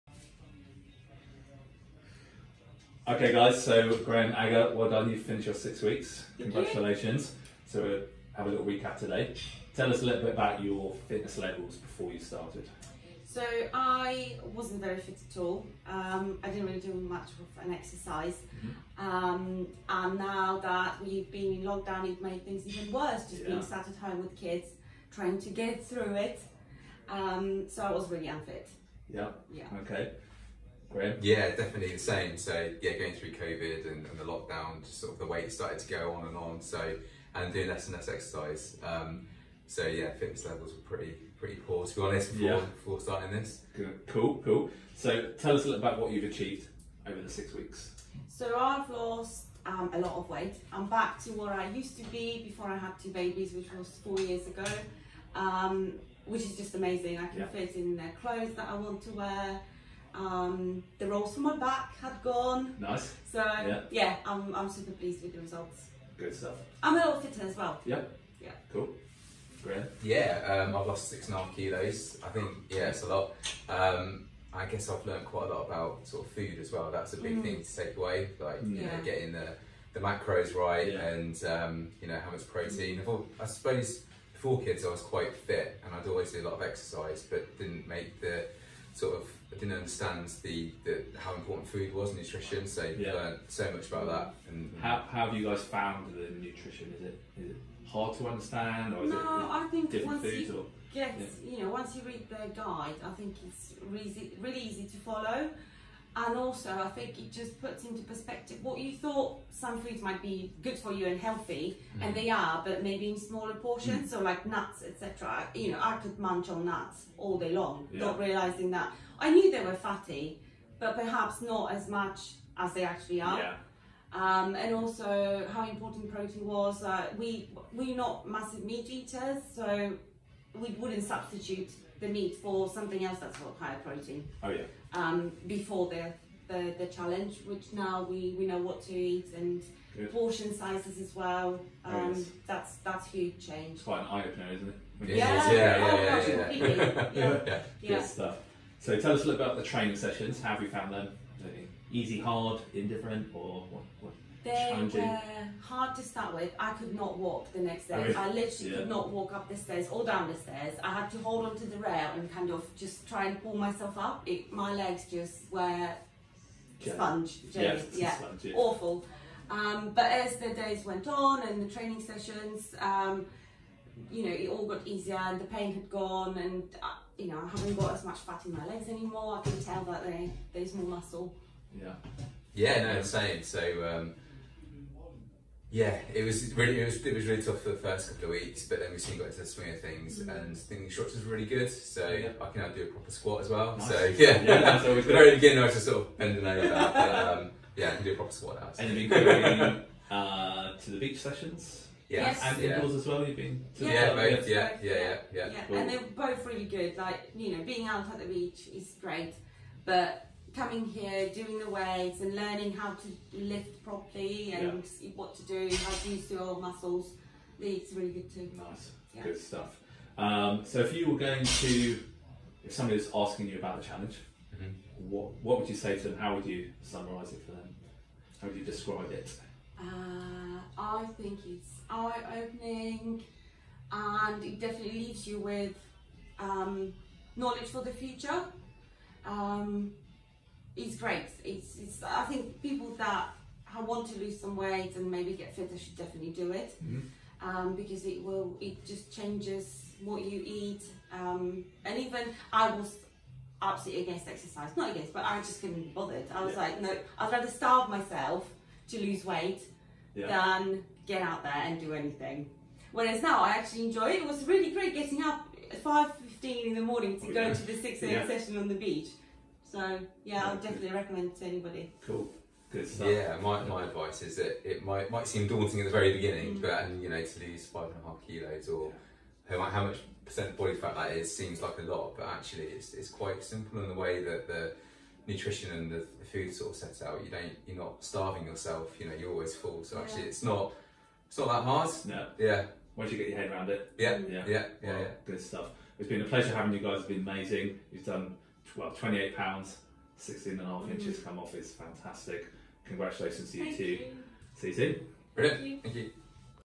Graham and Aga - 6 week challenge at 180 Strength
🙌 🙌 how about this - these two lots a whopping 28lbs between them, a stone each! Smashed it. Well done Graham and Aga!